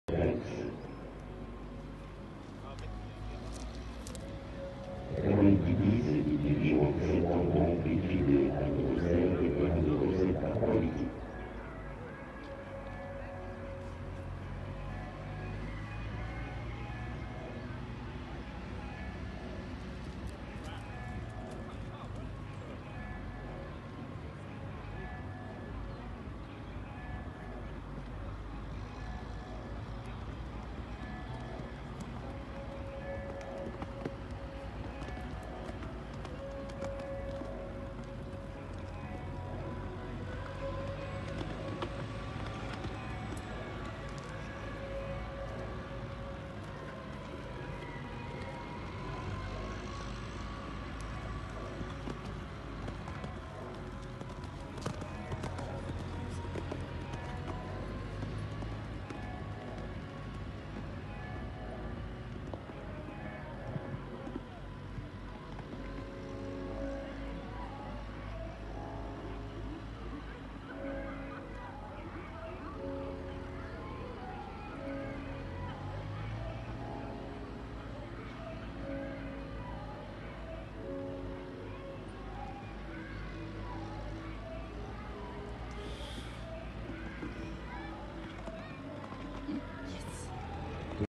Aquality victoire 7YH Villeneuve Loubet 2019
Victoire dans les 7aYH à Villeneuve Loubet pour A Quality d'I Z, par Amiro Z et Ramiro Z. La jument est montée par Cyril Benet. Plus sous